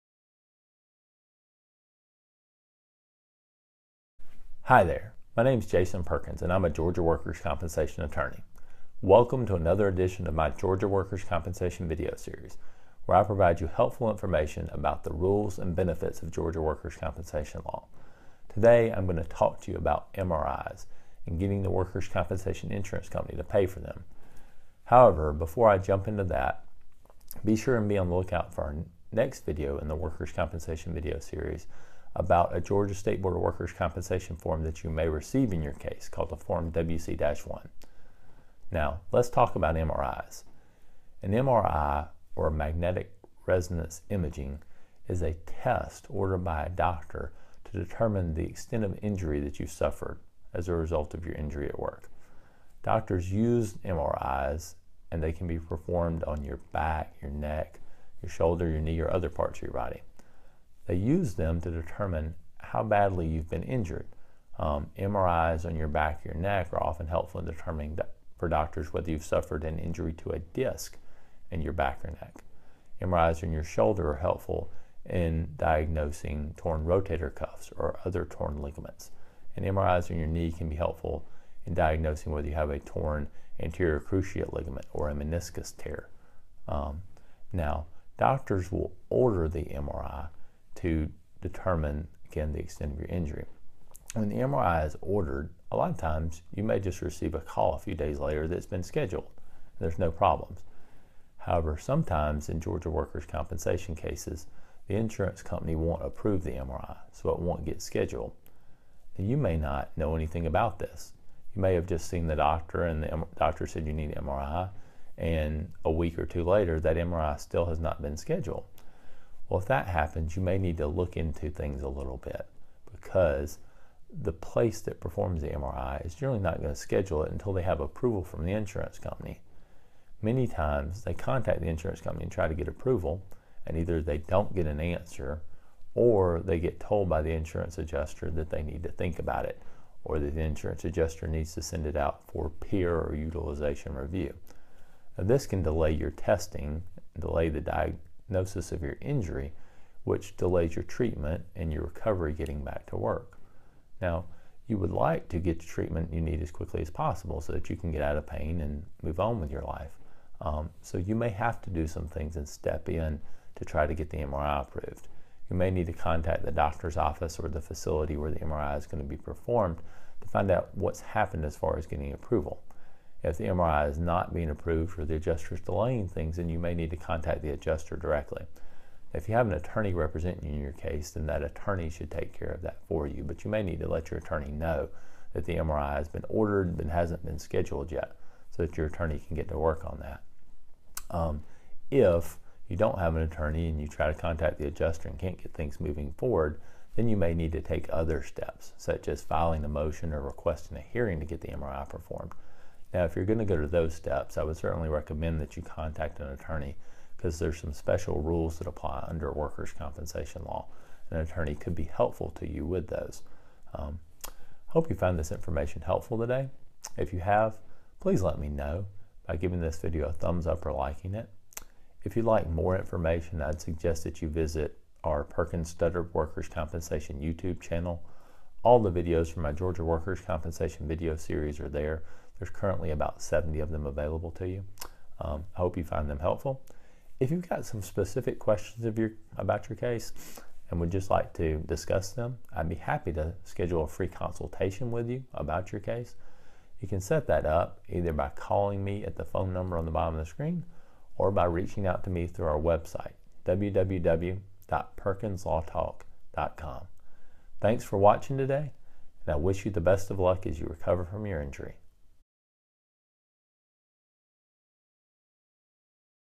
Making Workers' Compensation Pay For An MRI
An MRI is a diagnostic test ordered by a doctor to diagnose your medical condition.  Orthopedic doctors often order MRIs in workers' compensation claims to diagnose serious neck, back, shoulder, and knee injuries.  Georgia workers' compensation attorney Jason Perkins discusses how to make sure your MRI gets approved so that you can recover from your injury in this episode of the Georgia Workers' Compensation Video Series.